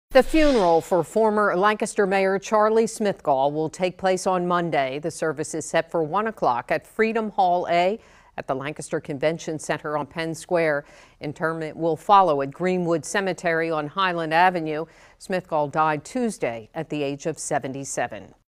Former Lancaster Mayor Charlie Smithgall's funeral set for Monday
The funeral for former Lancaster Mayor Charlie Smithgall will take place Monday at the Lancaster County Convention Center.